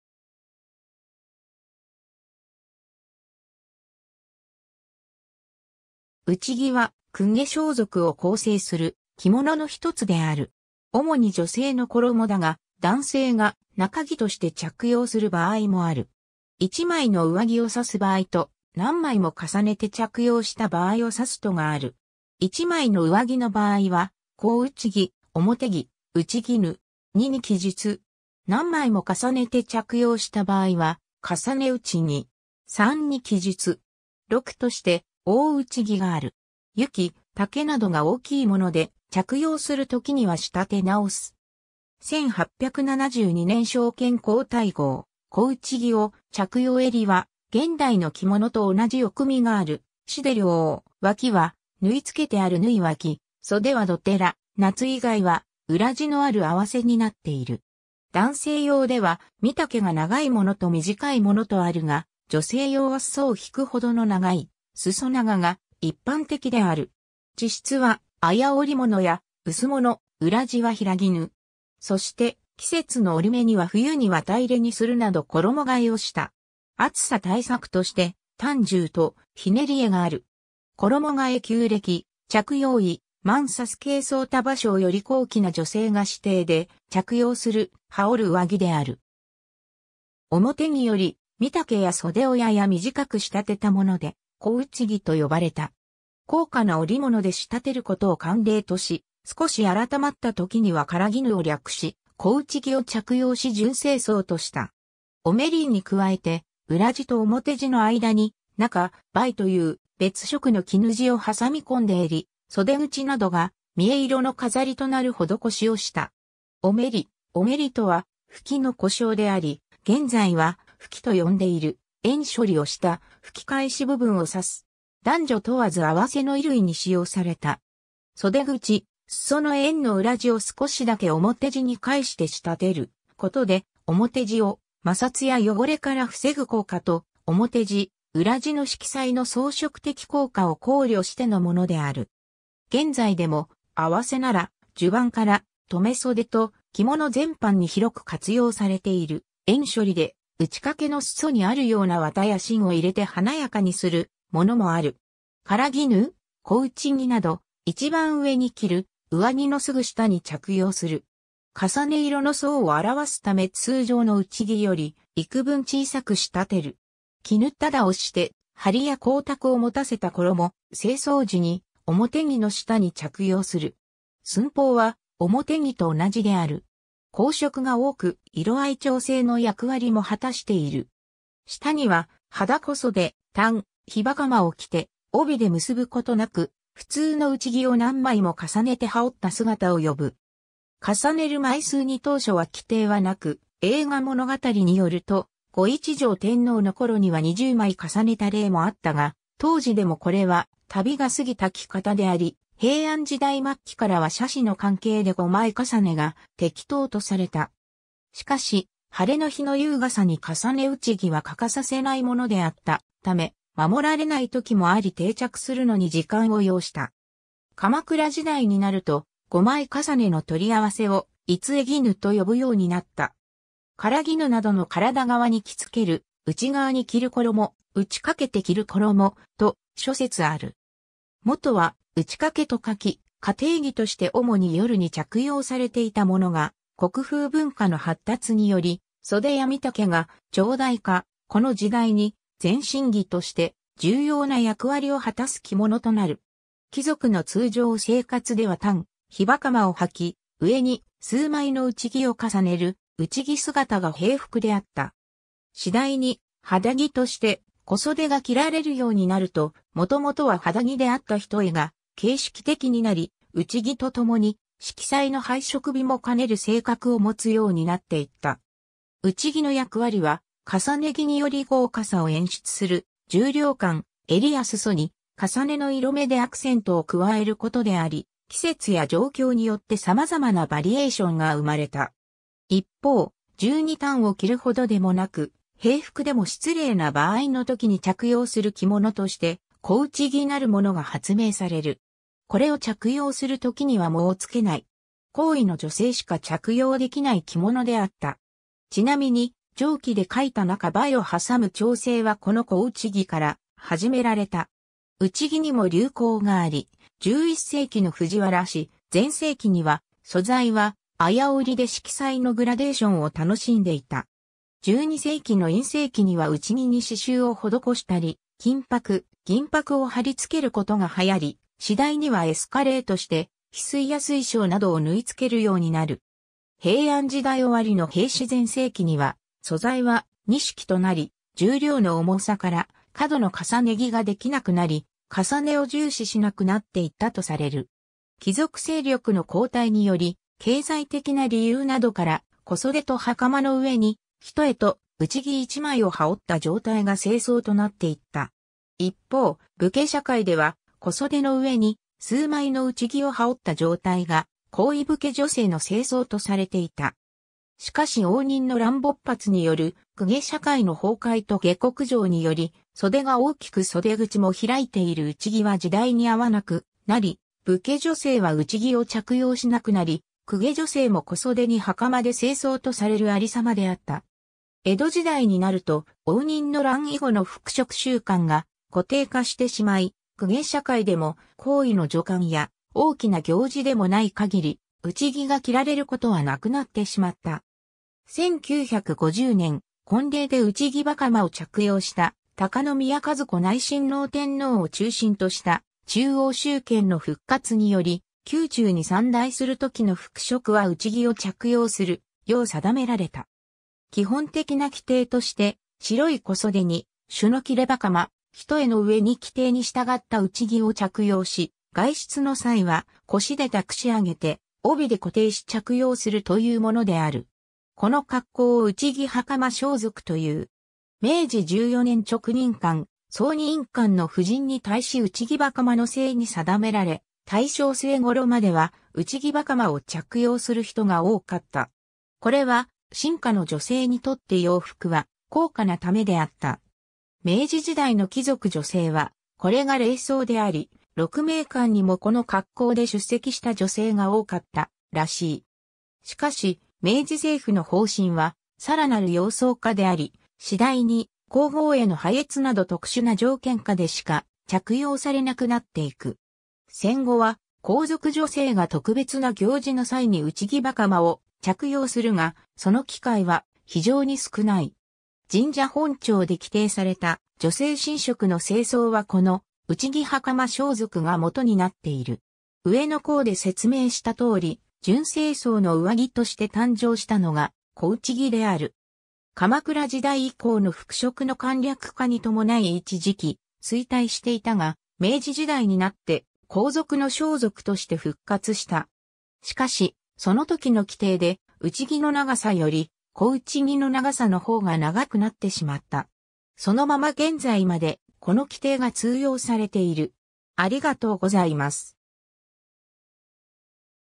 袿
宮廷・公家装束
袿（うちき、うちぎ）は、公家装束を構成する着物の一つである。
主に女性の衣だが、男性が中着として着用する場合もある。
 一枚の上着を指す場合と、何枚も重ねて着用した場合を指すとがある。
 一枚の上着の場合は「小袿（こうちぎ）」・「表着（うわぎ）」・「打衣（うちぎぬ）」。
2に記述。
 何枚も重ねて着用した場合は「重ね袿（袿姿）」。
3に記述。
 禄（下賜品）として「大袿」がある。
裄・丈などが大きいもので、着用する時には仕立て直す。
 1872年昭憲皇太后、小袿を着用 襟は現代の着物と同じ衽（おくみ）がある垂領（たりくび）、脇は縫付けてある縫腋（ほうえき）、袖は広袖、夏以外は裏地のある袷（あわせ）になっている。
 男性用では身丈が長いものと短いものとあるが、女性用は裾を引くほどの長い裾長が一般的である。
 地質は綾織物や薄物、裏地は平絹。
そして、季節の折目には冬に綿入れにするなど衣替えをした。
暑さ対策として「単重（ひとえがさね）」と「捻り重（ひねりがさね）」がある。
 —『衣替え』　旧暦・着用衣、「満佐須計装束抄」より 高貴な女性が私邸で着用する、羽織る上着である。
表着より身丈や袖をやや短く仕立たもので、小袿と呼ばれた。
高価な織物で仕立てることを慣例とし、少し改まった時には唐衣を略し、小袿を着用し準正装とした。
 おめりに加えて裏地と表地の間に中陪（なかべ）という、別色の絹地を挟み込んで衿・袖口などが三重色の飾りとなる施しをした。
 おめり（または「ふき・袘 」） おめりとは「ふき」の古称であり、現在はふきと呼んでいる縁処理をした、吹き返し部分を指す。
男女問わず袷の衣類に使用された。
 袖口・裾の縁の裏地を少しだけ表地に返して（ずらして）仕立てることで、表地を摩擦や汚れから防ぐ効果と、表地・裏地の色彩の装飾的効果を考慮してのものである。
 現在でも、袷なら襦袢から留袖と着物全般に広く活用されている縁処理で、打掛の裾にあるような綿や芯を入れて華やかにするものもある。
 唐衣（からぎぬ）・小袿など、一番上に着る上着のすぐ下に着用する。
重ね色の層を表すため通常の袿より幾分小さく仕立てる。
 砧打（きぬたうち）をして張りや光沢をもたせた衣、正装時に表着の下に着用する。
寸法は表着と同じである。
紅色が多く、色合い調整の役割も果たしている。
 下には肌小袖・単・緋袴（ひのはかま）を着て、帯で結ぶことなく普通の袿を何枚も重ねて羽織った姿を呼ぶ。
重ねる枚数に当初は規定はなく、『栄花物語（巻第二十四）』によると後一条天皇の頃には二十枚重ねた例もあったが、当時でもこれは度が過ぎた着方であり、平安時代末期からは奢侈の関係で五枚重ねが適当とされた。
しかし、晴れの日の優雅さに重ね袿は欠かさせないものであったため、守られない時もあり定着するのに時間を要した。
 鎌倉時代になると、五枚重ねの取り合わせを「五衣（いつつぎぬ）」と呼ぶようになった。
 唐衣などの身体側に着付ける「内側に着る衣」「打ち掛けて着る衣」、と諸説ある。
 元は「袿衣」と書き、家庭着として主に夜に着用されていたものが、国風文化の発達により、袖や身丈が長大化、この時代に全身着として重要な役割を果たす着物となる。
 貴族の通常生活では単、緋袴を穿き、上に数枚の袿を重ねる袿姿が平服であった。
次第に肌着として小袖が着られるようになると、もともとは肌着であった単が形式的になり、袿と共に色彩の配色美も兼ねる性格を持つようになっていった。
 袿の役割は重ね着により豪華さを演出する重量感、衿や裾に襲の色目でアクセントを加えることであり、季節や状況によって様々なバリエーションが生まれた。
 一方、十二単を着るほどでもなく、平服でも失礼な場合の時に着用する着物として小袿なる物が発明される。
これを着用するときには裳をつけない。
高位の女性しか着用できない着物であった。
ちなみに上記で書いた中陪をはさむ調製はこの小袿から始められた。
 袿にも流行があり、11世紀の藤原氏全盛期には素材は綾織で色彩のグラデーション（襲）を楽しんでいた。
12世紀の院政期には袿に刺繍をほどこしたり、金箔・銀箔を張り付けることが流行り、次第にはエスカレートしてヒスイや水晶などを縫いつけるようになる。
平安時代終わりの平氏全盛期には素材は錦となり、重量の重さから過度の重ね着ができなくなり、襲を重視しなくなっていったとされる。
 貴族勢力の後退により、経済的な理由などから小袖と袴の上に単と袿一枚を羽織った状態が正装となっていった。
 一方、武家社会では...